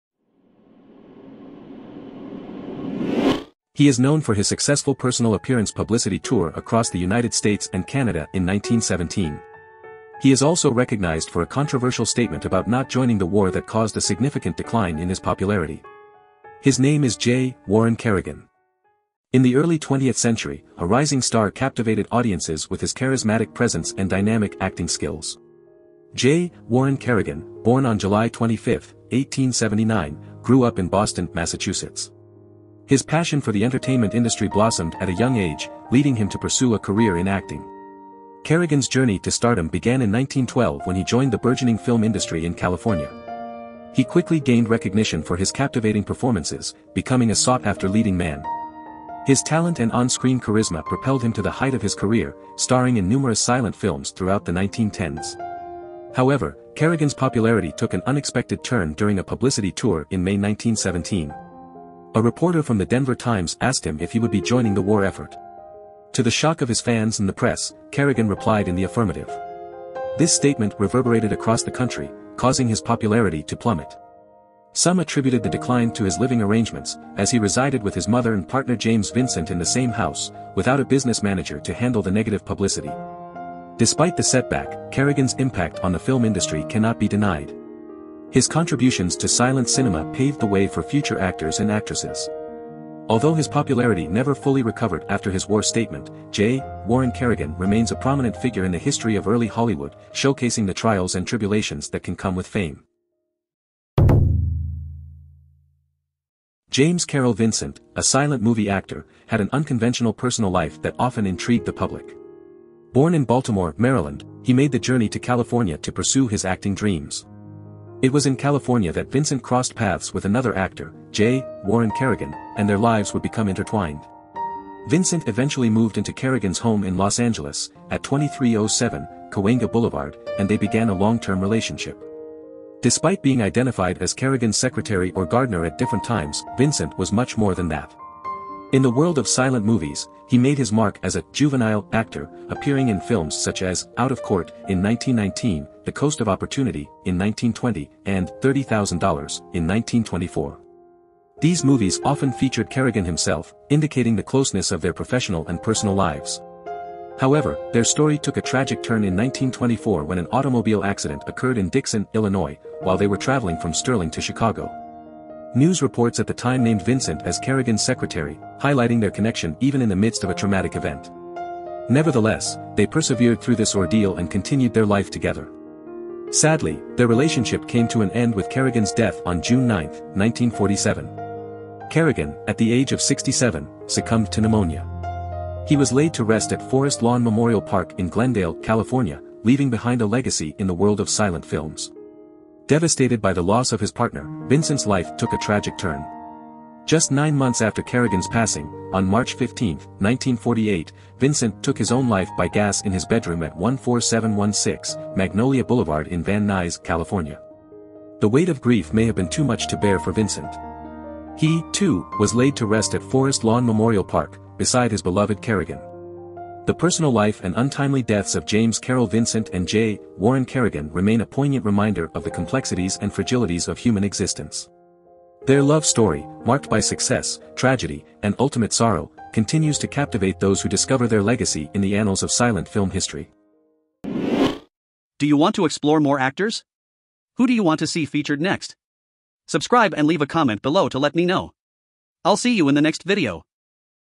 J. Warren Kerrigan: From Stardom to Controversy | Actors & Actresses Biography
James Carroll Vincent was a silent movie actor. He was born in Baltimore, Maryland, and…